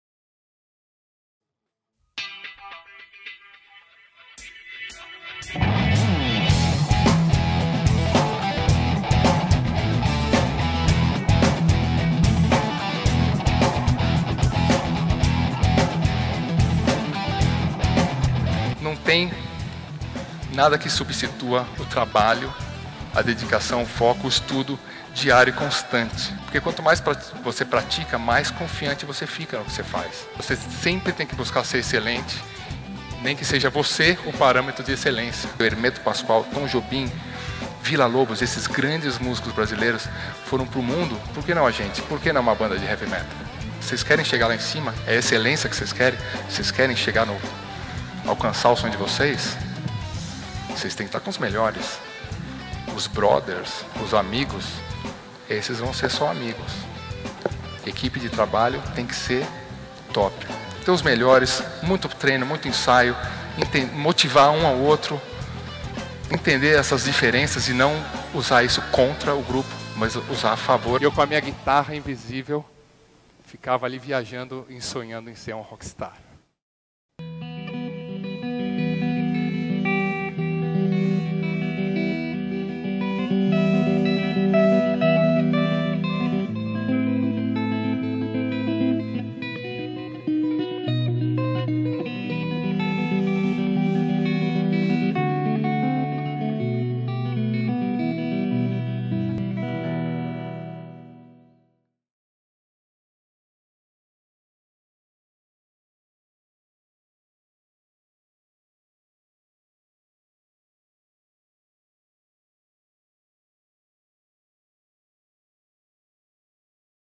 Top 10 Empresarial - Kiko Loureiro
Kiko Loureiro fechou 2014 com chave de ouro na noite do Top 10 Empresarial de novembro.